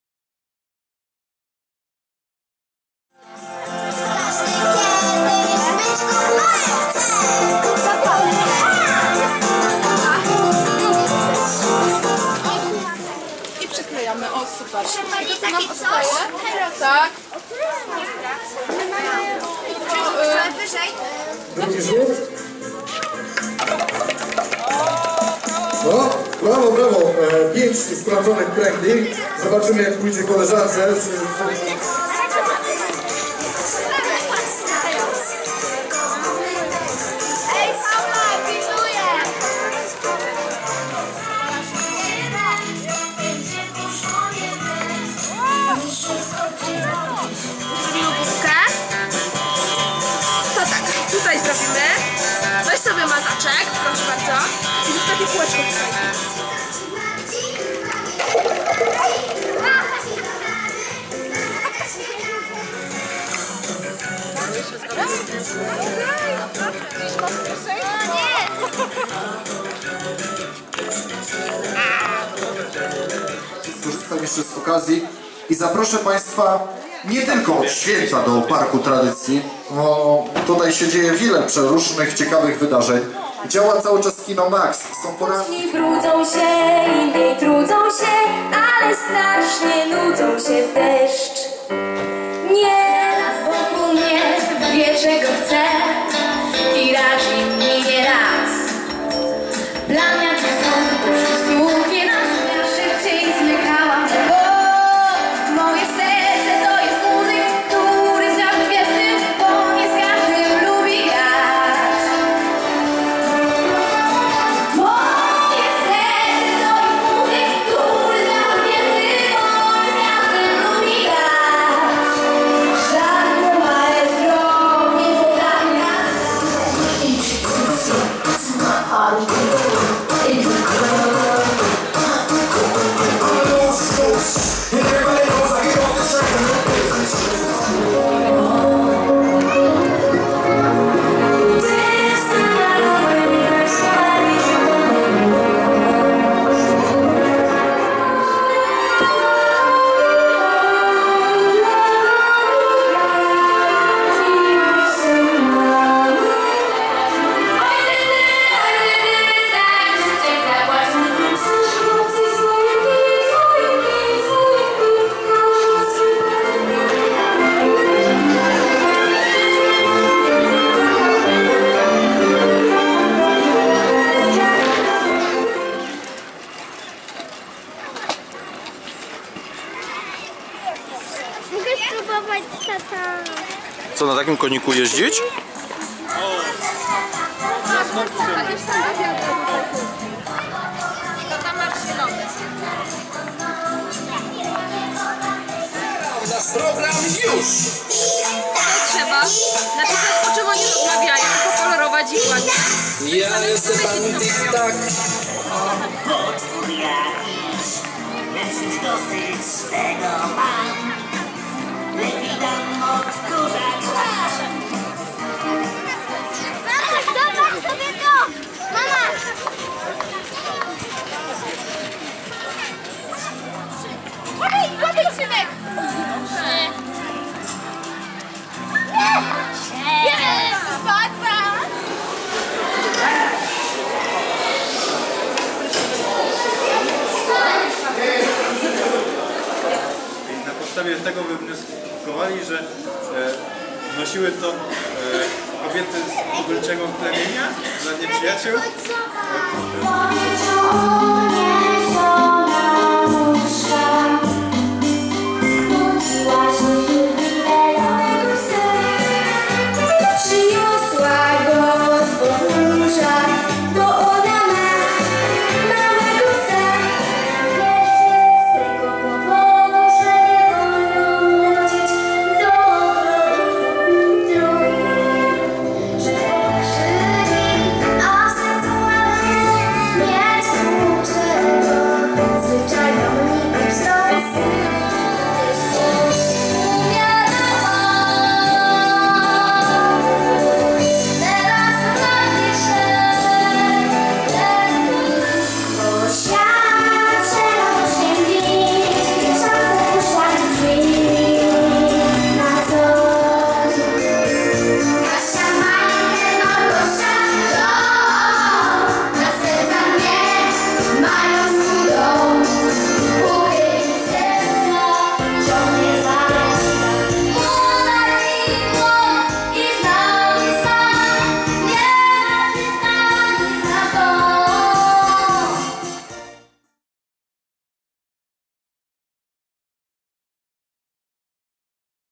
Dzień bajtla w SCK Parku Tradycji w Michałkowicach - 01.06.2013r.
Film oglądaj w jakości Full HD

Relacja z Dnia bajtla w SCK Parku Tradycji w Michałkowicach